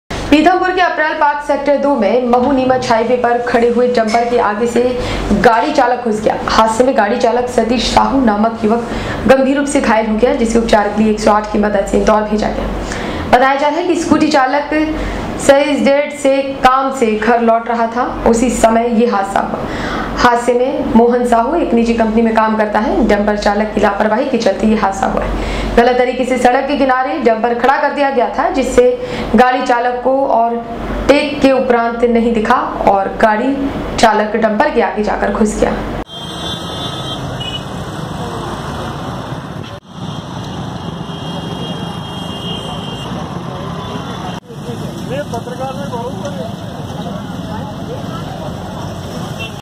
पिथमपुर के अप्रैल पार्क सेक्टर 2 में महू नीमच हाईवे पर खड़े हुए डंपर के आगे से गाड़ी चालक घुस गया
पिथमपुर के अप्रैल पार्क सेक्टर 2 में महू नीमच हाईवे पर खड़े हुए डंपर के  आगे से गाड़ी चालक घुस गया हादसे में गाड़ी चालक सतीश  साहू नामक युवक गंभीर रूप से घायल हो गया जिसे उपचार के लिए 108 की मदद से इंदौर भेजा गया है बताया जा रहा है के स्कूटी चालक एसईजेड से काम से घर लौट रहा था उसी समय यहां हादसा हुआ, हादसे मोहन साहू एक निजी कंपनी में काम करता है, डंपर चालक की  लापरवाही के चलते यहाँ हादसा हुआ हैं , गलत तरीके से  सड़क के किनारे डंफर खड़ा कर दिया था जिसे गाड़ी चालक को और टेक के उपरांत नही दिखा और गाड़ी चालक डंफर के आगे जाकर घुस गया ।